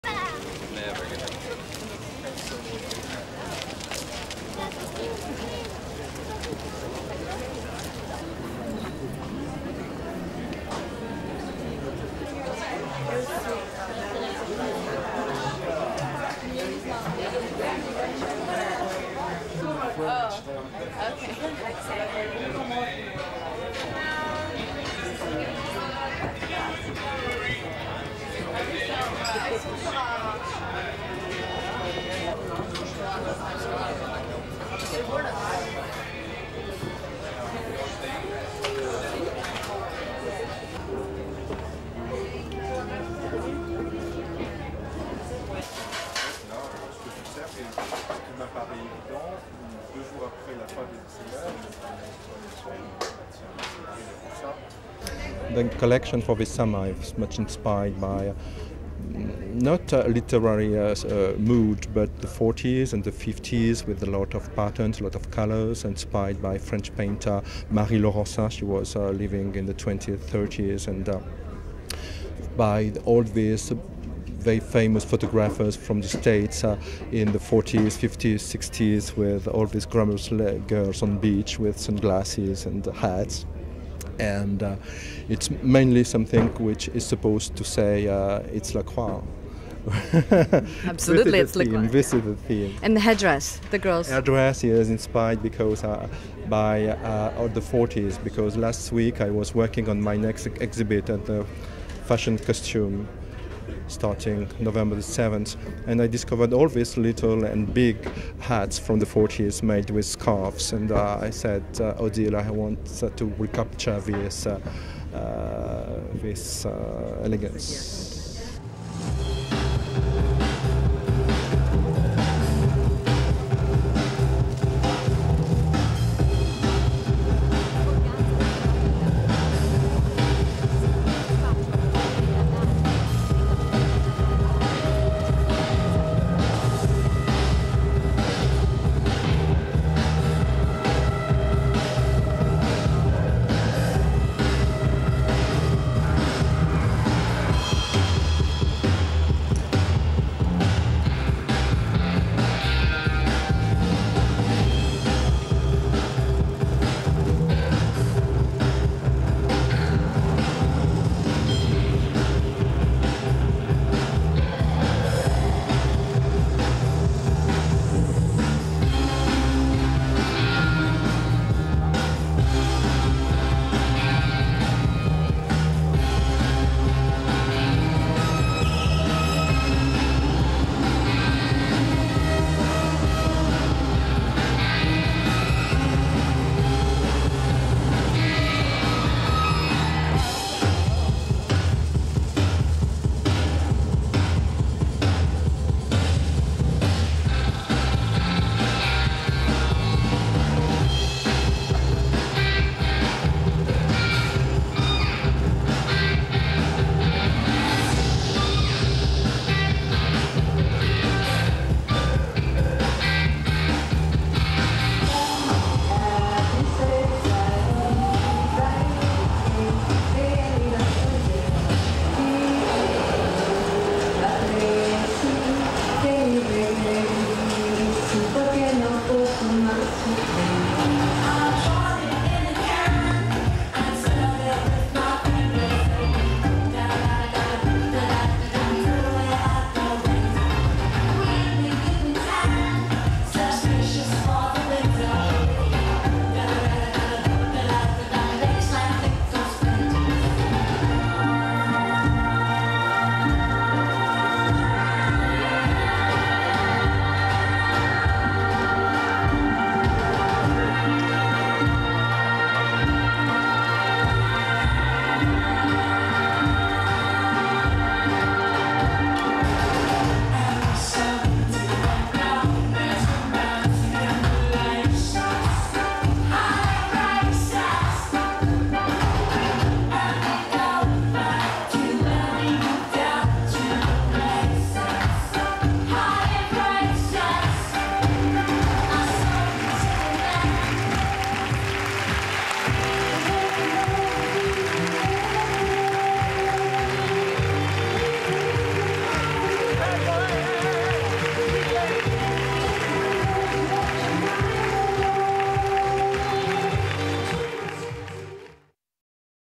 A riot of colour at Lacroix spring ready to wear collection
1. Various exteriors of venue
2. Various of backstage preparations
3. Medium pull out to wide of Christian Lacroix, designer
4. SOUNDBITE (English) Christian Lacroix, designer:
"The collection for this summer was inspired not by a literary mood but (by) the 40's and the 50's with a lot of patterns, a lot of colours. Inspired by French painter Mary Laurencin. She was living in the 20's and 30's. (It was inspired) By all the these very famous photographers from the states, in the 40's 50's(and) 60's with all these glamorous girls on the beach with sunglasses and hats. It's mainly something which is supposed to say, 'It's Lacroix'".
Reporter Q: "And the head-dress?"
Christian Lacroix: "Yes, inspired by the 40's because last week I was working on my exhibit at the Fashion Costume, starting November 7th and I discovered all these little and big hats from the 40's made with scarves and I said, Odile, (referring to his hat designer) I want to recapture this elegance."
5. Highlights from catwalk
A RIOT OF COLOUR AT LACROIX
Lacroix let loose a riot of colour at his spring ready-to-wear show in Paris, with bustier dresses in retro floral prints or brushstroke patterns set off by jaunty cartwheel hats and straw boaters covered in silk scarves.
The first part of the show contained some more low key numbers, including graphic coats in black-and-white tweed, pencil skirts and sedate belted tops, as well as a lineup of classic little black dresses.
But midway through the show, the more exuberant trademark Lacroix style shone through with diaphanous gowns and glam gal swimwear, some detailed with faux pythons, tin and resin embroideries and African beading.
Accessories, included bright bracelets banded up the arms, colourful gloves that only covered the fingers and boldly printed head wraps topped with what looked like satellite dishes.
The designer said he would curate the annual Arles Photo Festival next summer and had been delving into images by 1940s American fashion photographers like Louise Dahl-Wolfe.